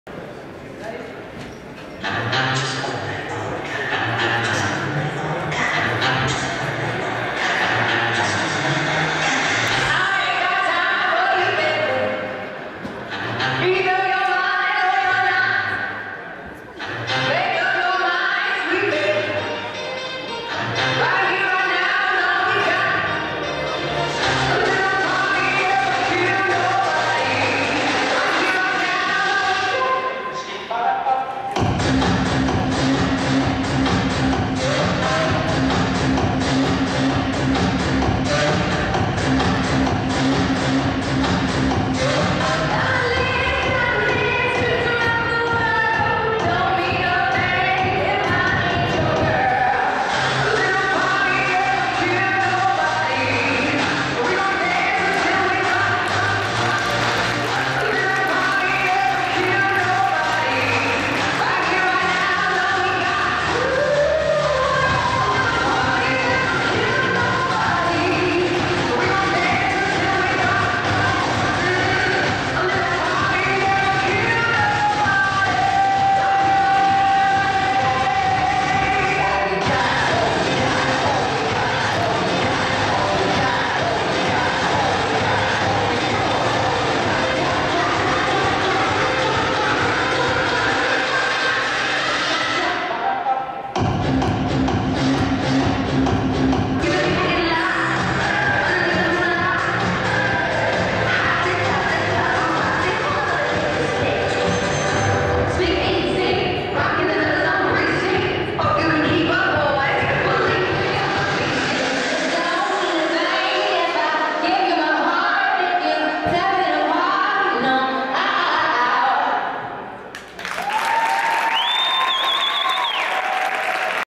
2013.12.17 SMC Dance Team Routine
The St. Mary's Colgan Dance Team performs at halftime of the basketball game versus McDonald County.